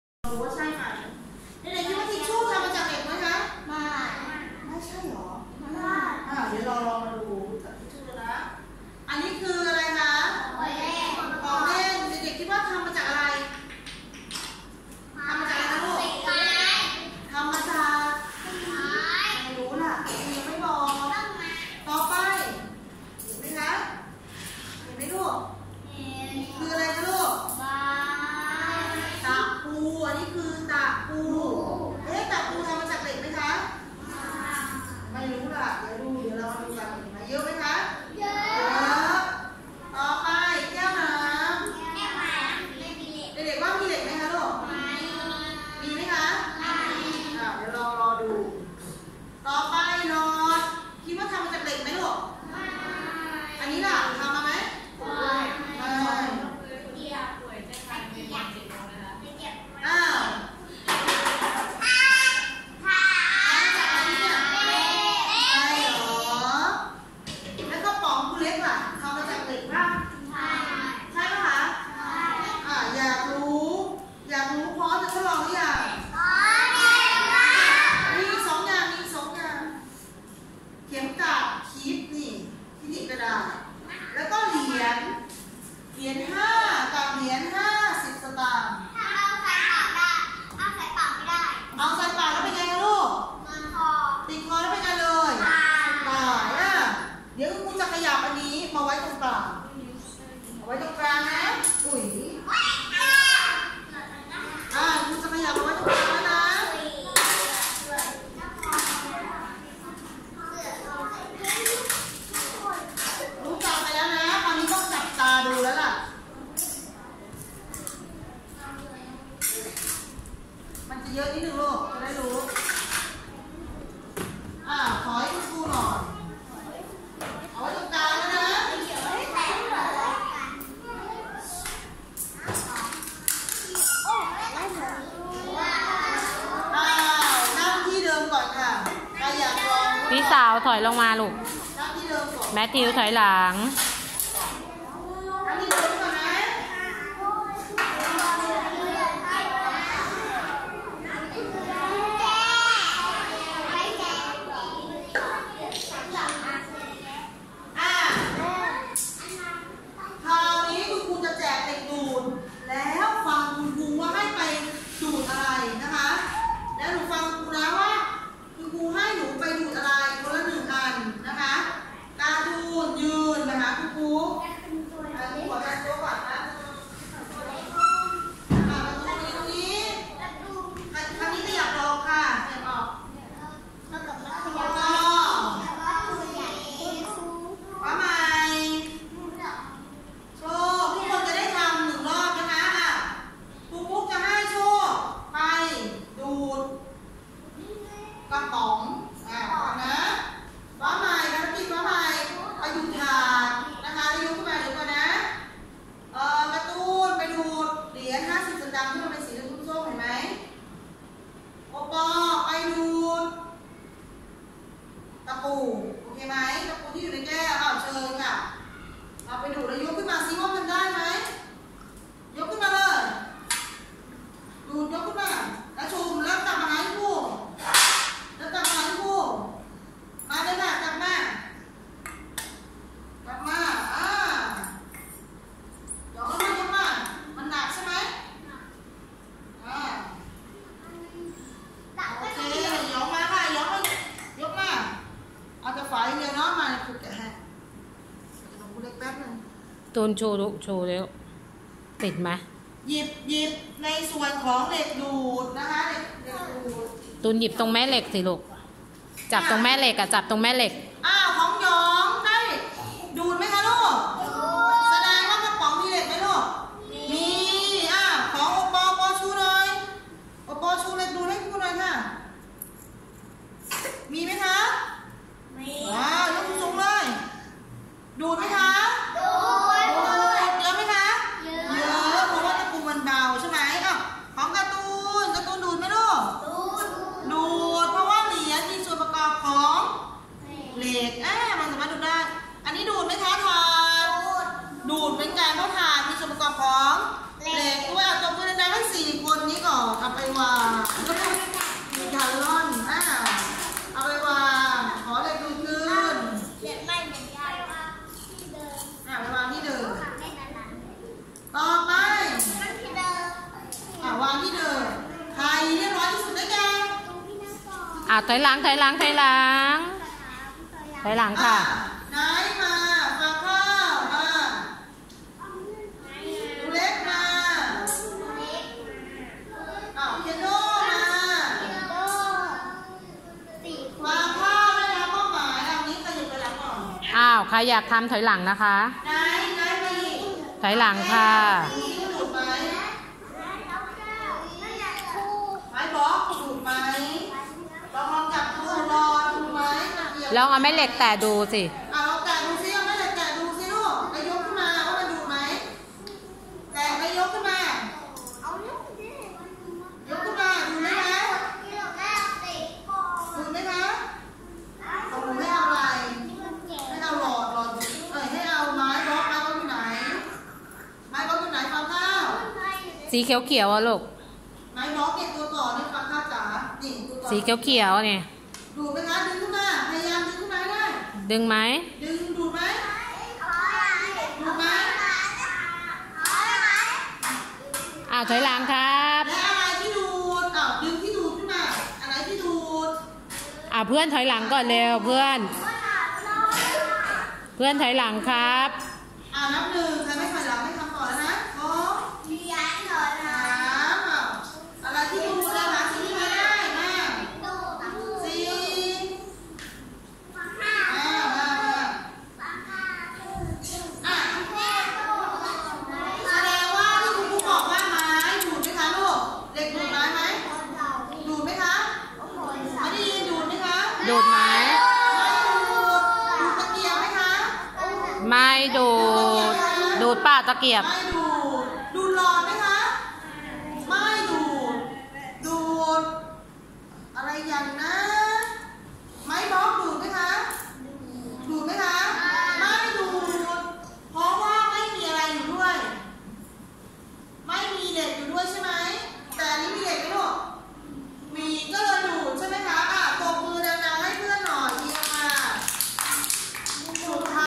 โรงเรียนคลองมหาสวัสดิ์ กิจกรรมการทดลอง วิทยา​ศาสตร์​ที่เด็กสามารถ​ทดลองด้วยตัวเองได้/61
เรื่อง​แม่เหล็ก​